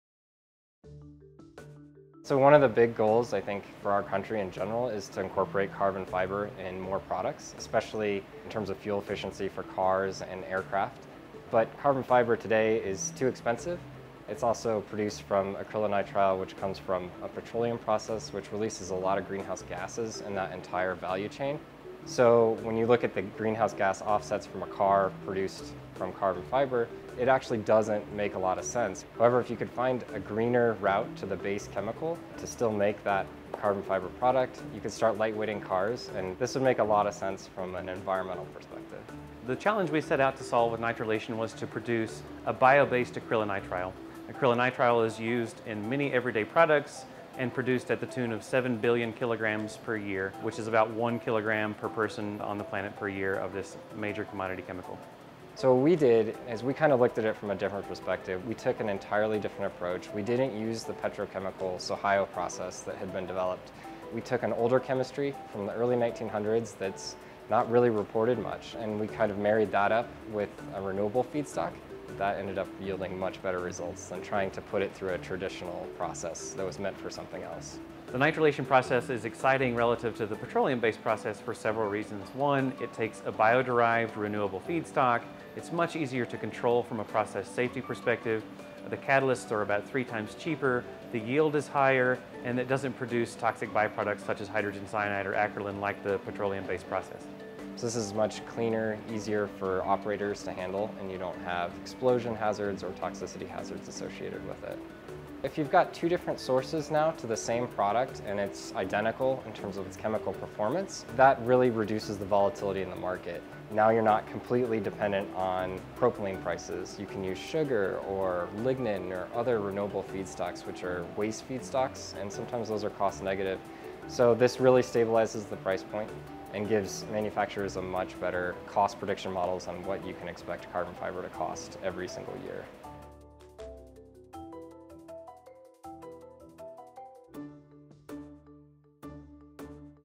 Making Carbon Fiber Renewable
Carbon fiber is a lightweight material for cars and planes but currently made with the chemical acrylonitrile that is produced from an energy-intensive process that produces hydrogen cyanide. Scientists discovered a groundbreaking new method that is a cheaper way to produce acrylonitrile, the building block of carbon fiber—and one immune from the swings of petroleum pricing.